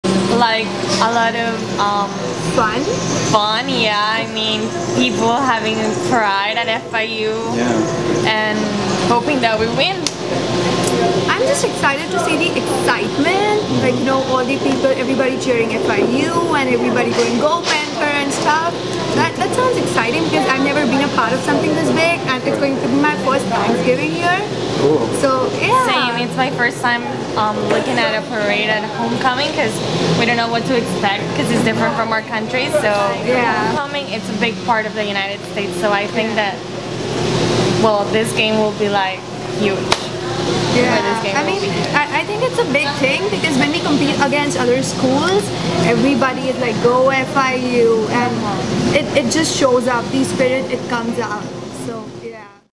International Students discuss Homecoming: Their 'first American experience."
International students Alejandra Villar, a sophomore majoring in international business and Yashaswi Tapadia, a freshman majoring in international business, discuss what they expect Homecoming to be like. Video by Contributing News Writer Jasiel Lopez/PantherNOW.